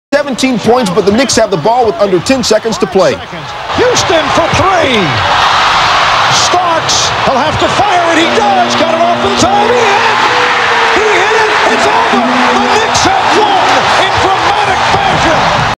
JOHN STARKS GAME WINNING WALK OFF BUZZER BEATER NY KNICKS VS THE SUNS AT MSG FEB 18 1997
JOHN STARKS GAME WINNING WALK OF BUZZER BEATER NY KNICKS VS THE SUNS AT MSG FEB 18 1997